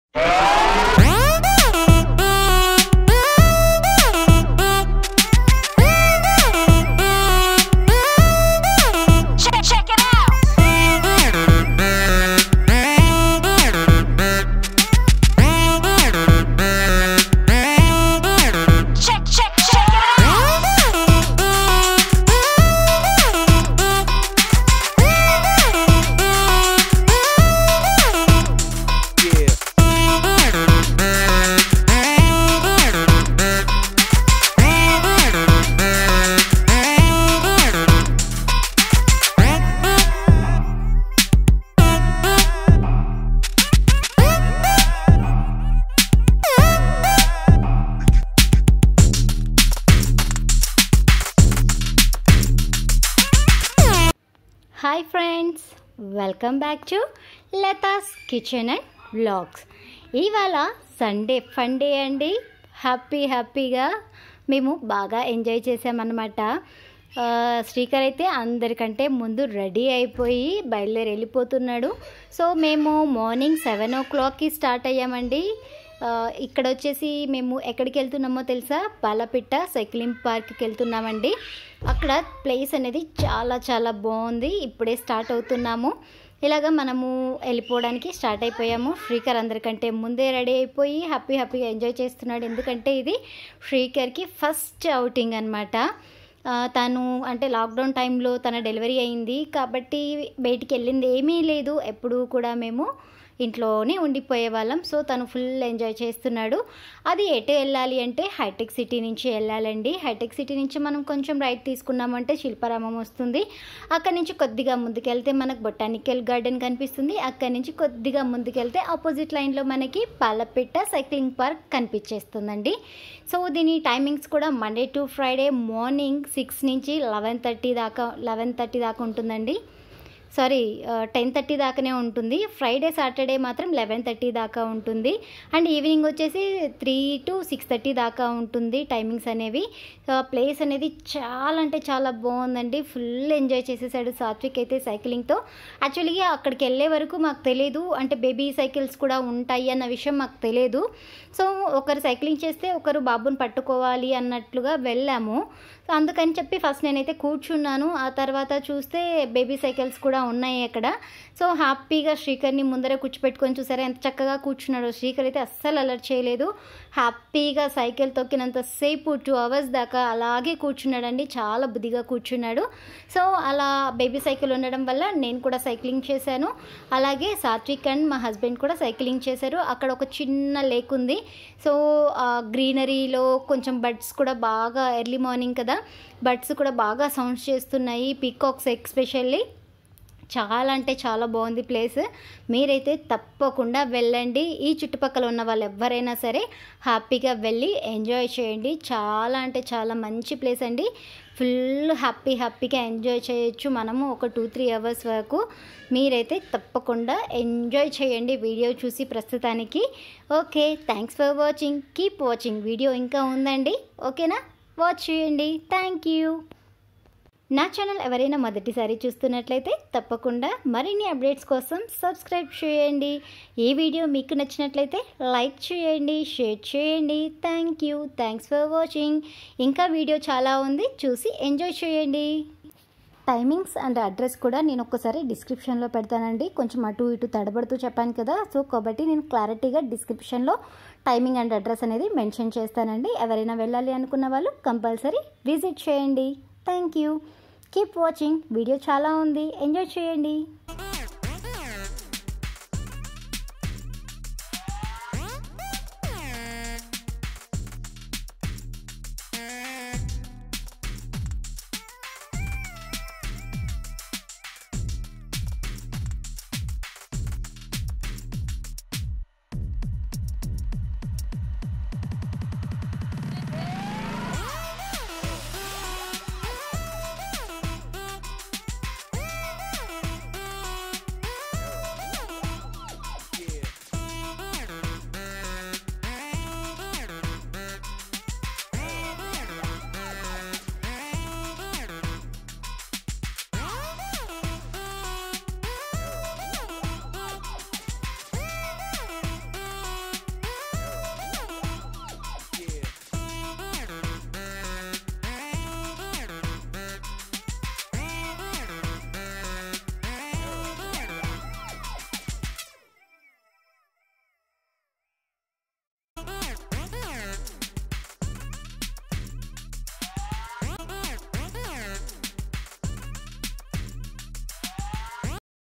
ఎంతో ఫన్ / Sunday is Funday / Srikar 1st Outing / Palapitta Cycling Park /Hyderabad Visiting places
Paala Pitta Cycling Park
Masjid Banda Road, Botanical Garden Rd, Camelot Layout, Gachibowli, Hyderabad, Telangana 500084

Timings :
Morning 6am - 10am
Evening 3pm - 6pm

Pala Pitta Cycling Park for short is a wonderful green urban infrastructure built by Telangana State Forest Development Corporation Ltd. (TSFDC) project to provide Cyberabad residents and tourists with safe, secure, and joyful cycling experiences. Named “Pala Pitta” (Blue Jay) after the State Bird of Telangana, this “Reserved Forest” is one of its kind in India, dedicated exclusively to cycling-based health and recreational activities.must watch this place its a super place for enjoyment, better to visit in the morning times. so many birds sounds and sun ray's etc.